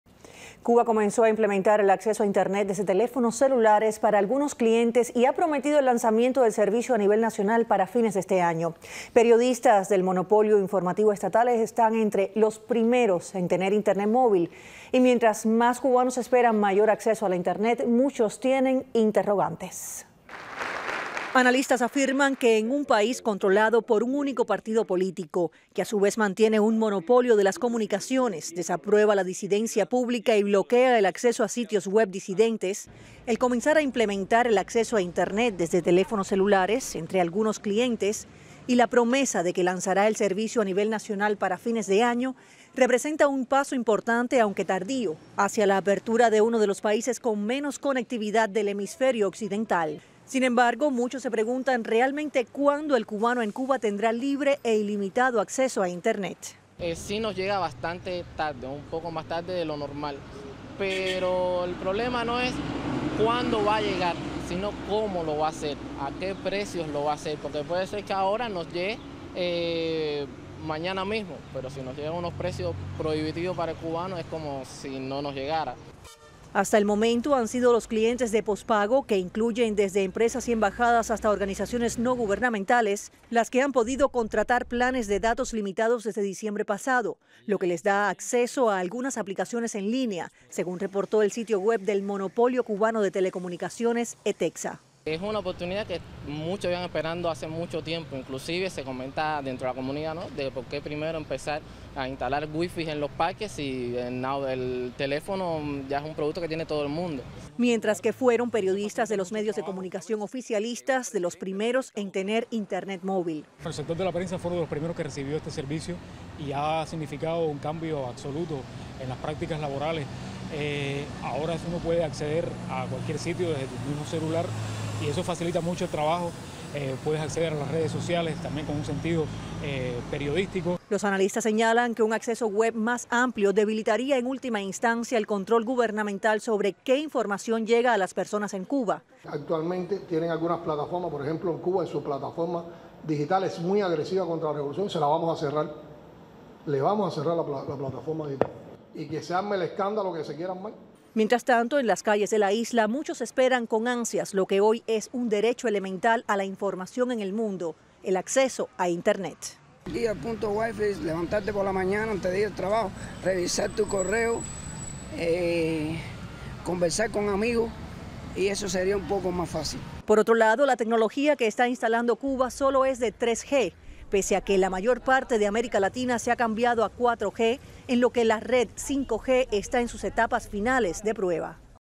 Gobierno cubano comienza a implementar acceso a internet desde celulares
El monopolio de las comunicaciones ETECSA comenzó a implementar el acceso a Internet desde teléfonos celulares entre algunos clientes y prometió el lanzamiento del servicio a nivel nacional para fines de año, lo que debilitaría el control gubernamental sobre la información.